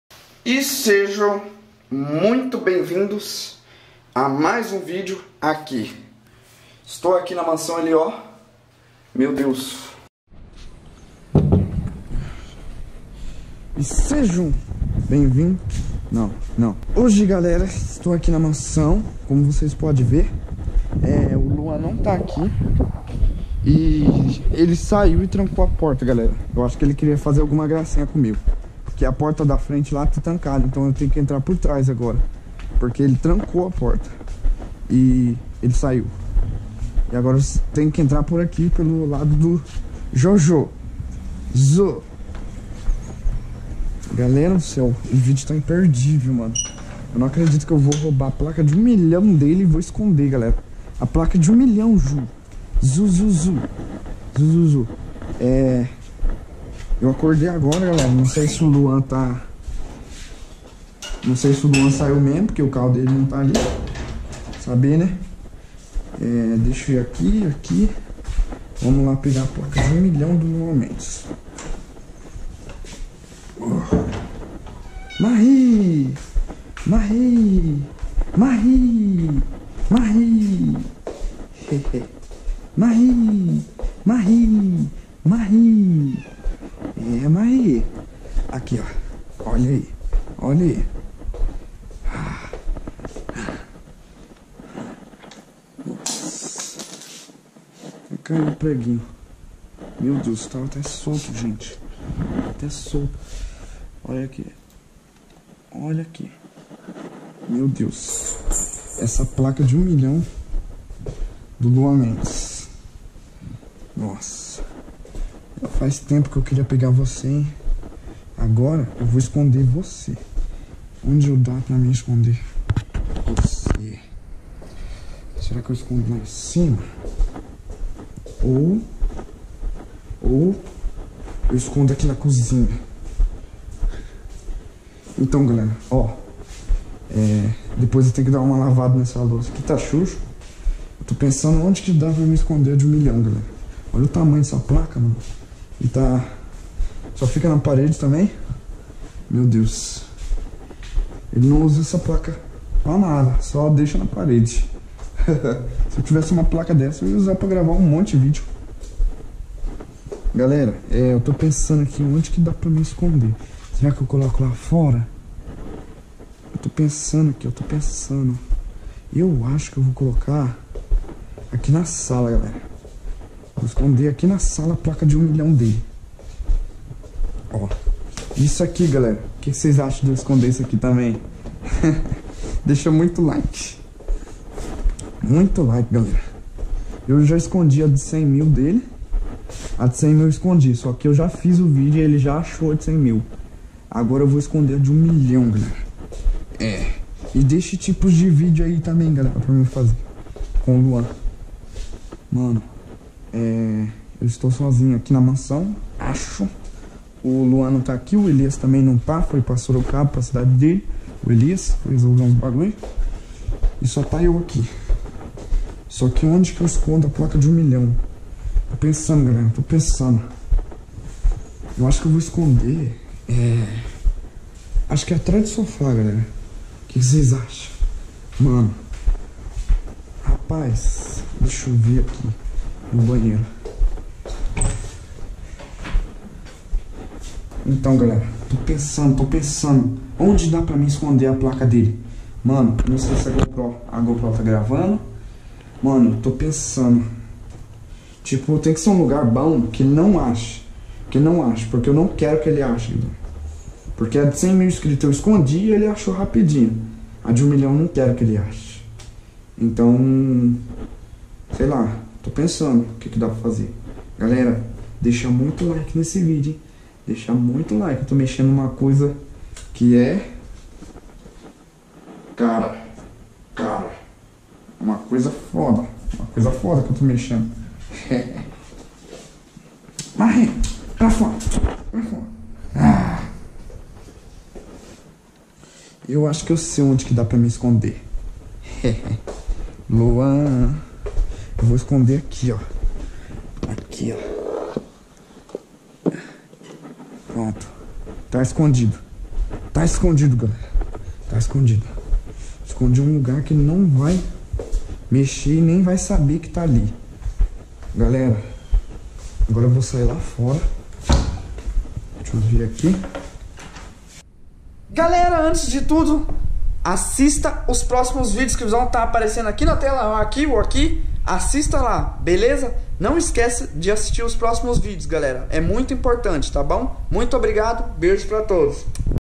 ESCONDI A PLACA DE 1 MILHÃO DE INSCRITO DO LUAN MENDES - MAIOR TROLLAGEM
.ME. SIGAAAAAA. AI.

●Contato para parcerias e eventos
SÓ CHAMA AQUI